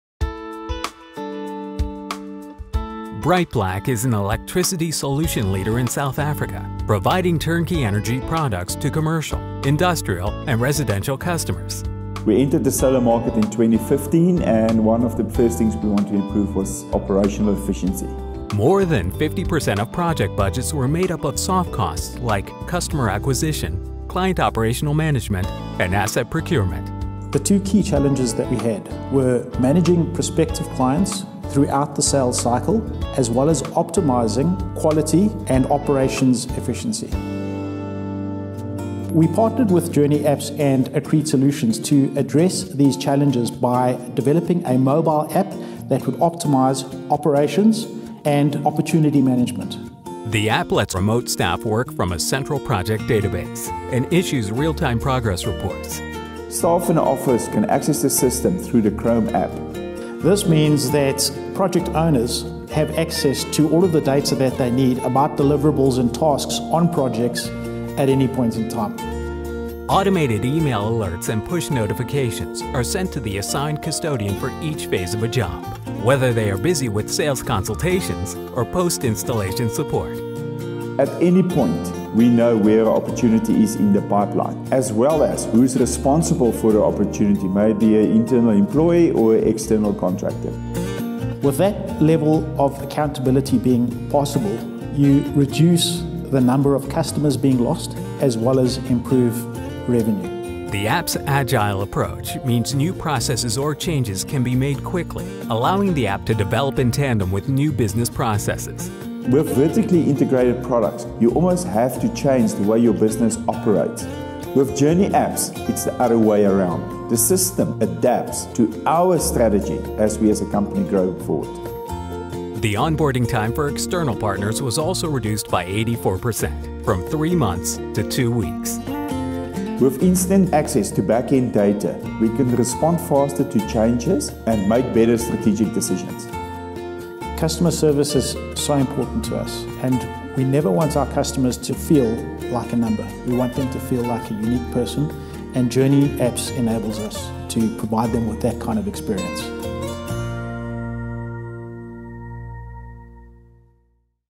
JourneyApps BrightBlack Case Study
A short case study video detailing how JourneyApps has helped BrightBlack to optimize their merchant sign-up process.

This video was produced, filmed, directed and edited by Wyrd Films in 2016.